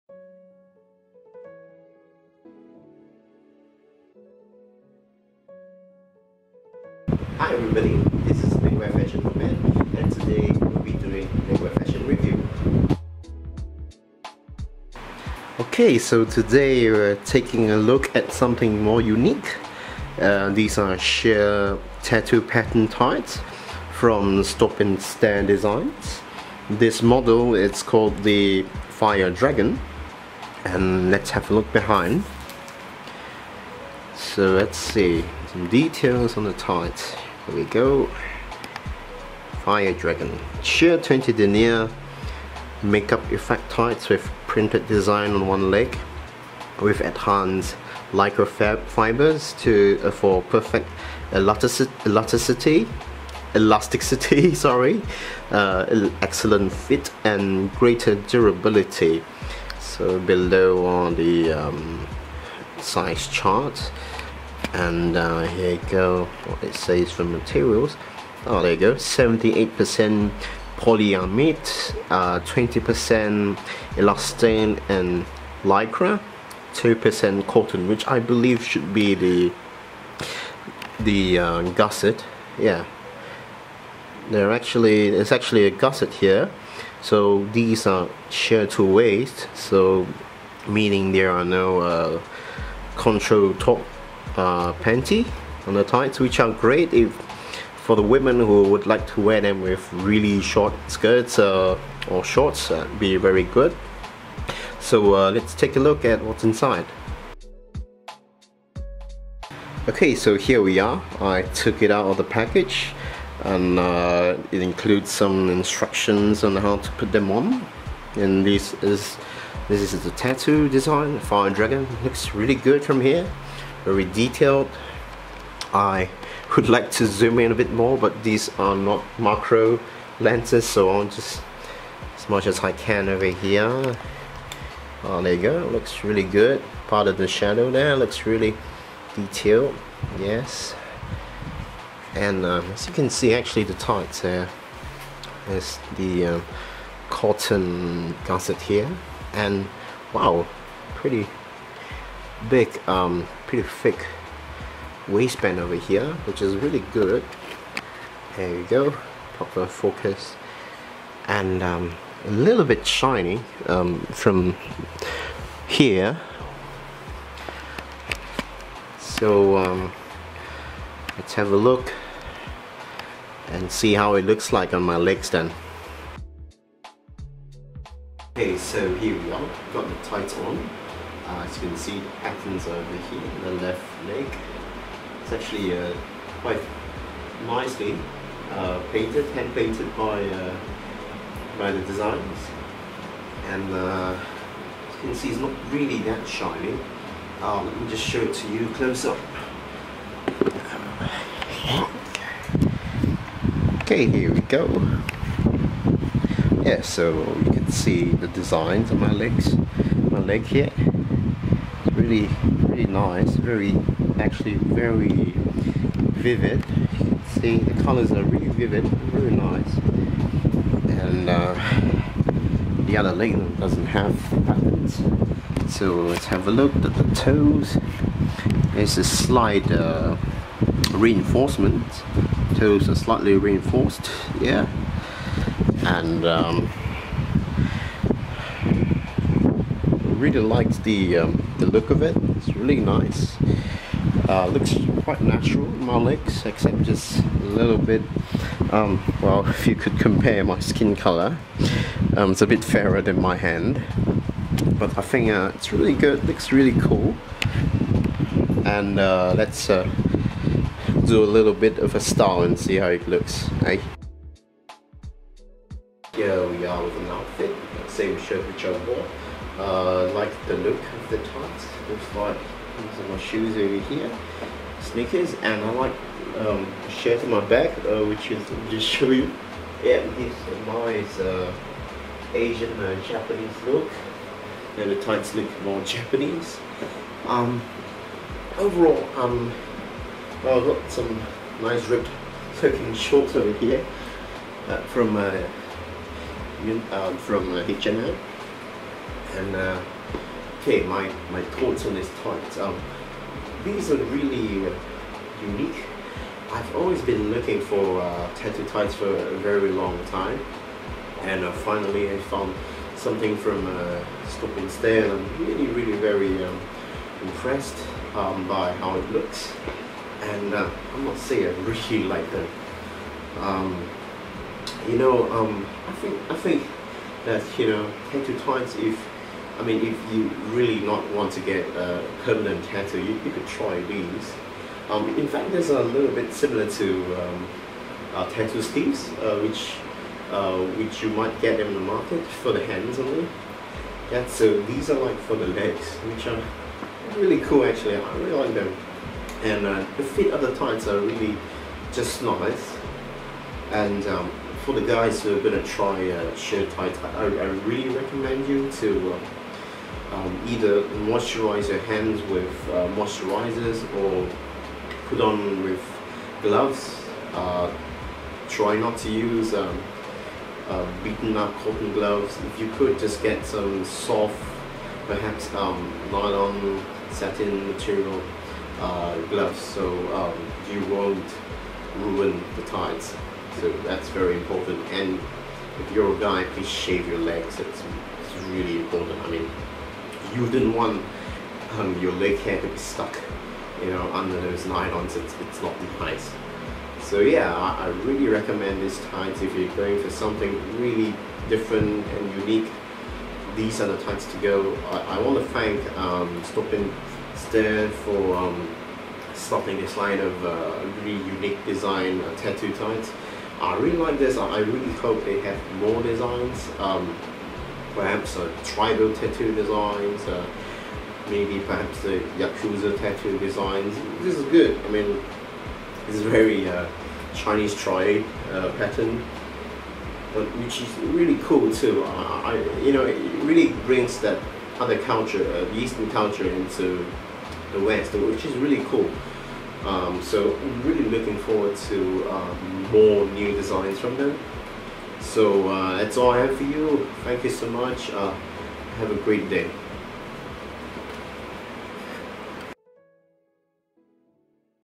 Legwear Fashion For Men: Stop and Stare Fire Dragon
Legwear Fashion For Men reviews Irish made Stop and Stare Fire Dragon Sheer Tattoo tights

Sorry for the windy sound! :(